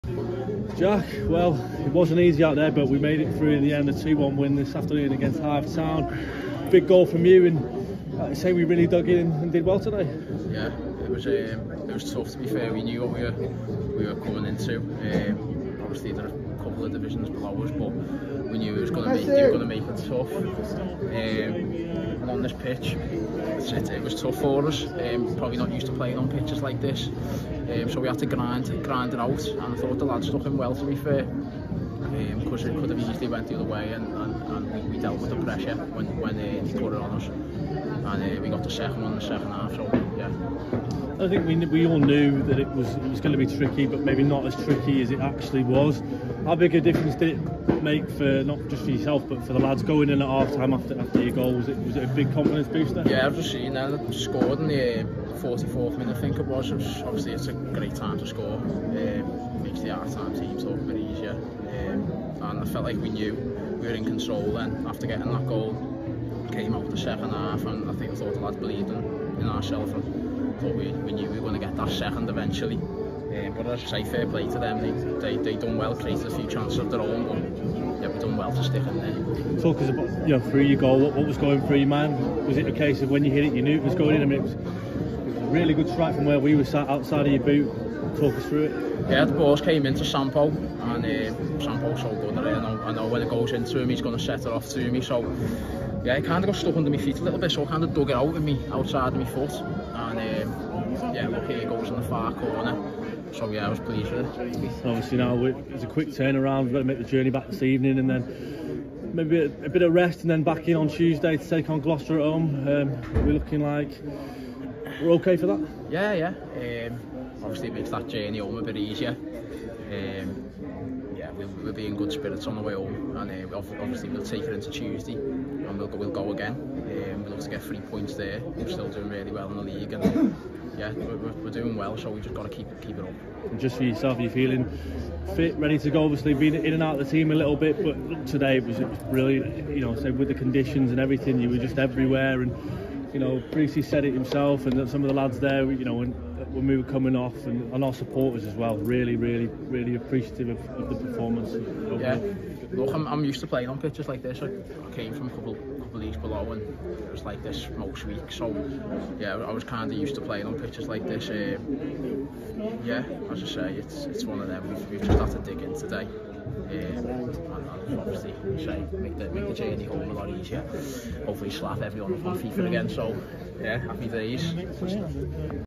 INTERVIEW: Jack Hazelhurst - Hythe Town vs Chorley
Pete Tscherewik speaks to Jack Hazlehurst after Chorley beat Hythe Town 1-2 to progress into the fifth round of the Isuzu FA Trophy